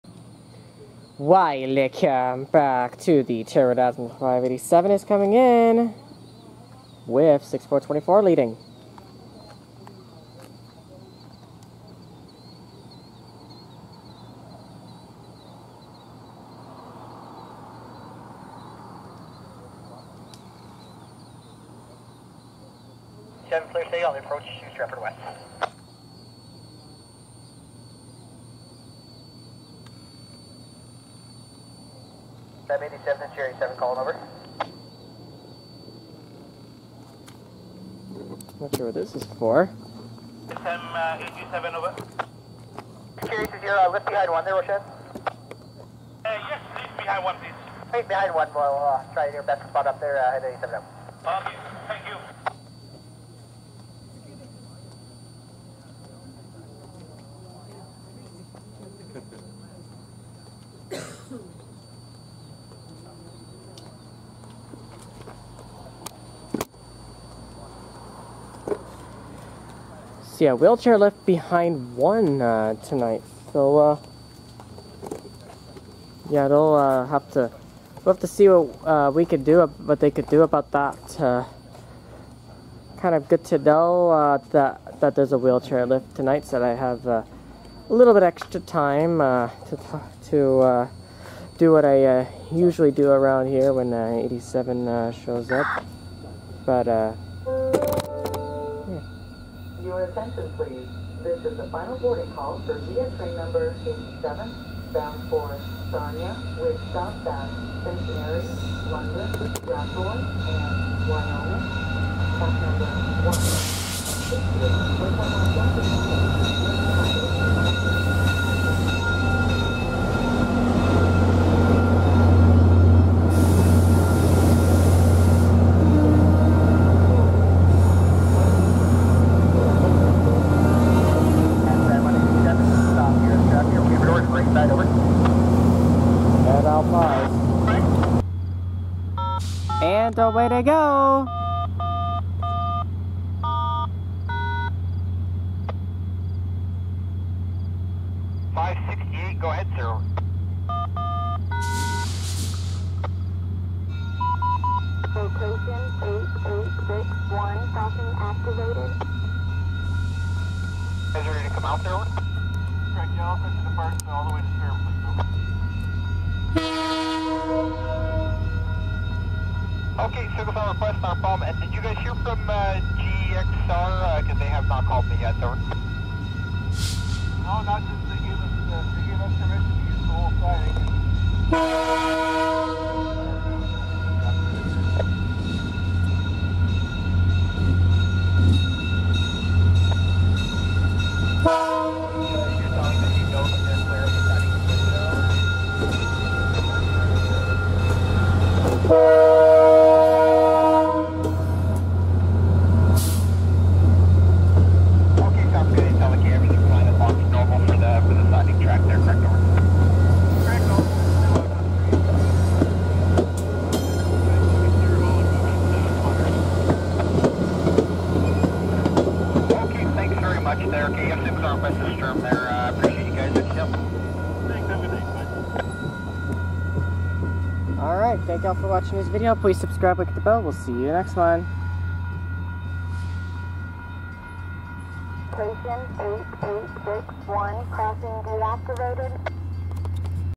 VIA 87 September 25, 2024
Train ID: VIA 87
Locomotive Number(s): 6424
Coach Numbers: 3472, 3334, 3302, 3301
Time: 20:00 EDT
Location: Stratford VIA Station (CN Stratford West)
Subdivision: Guelph
Milepost: 88.5
Origin: Toronto Union Station
Destination: Sarnia VIA Station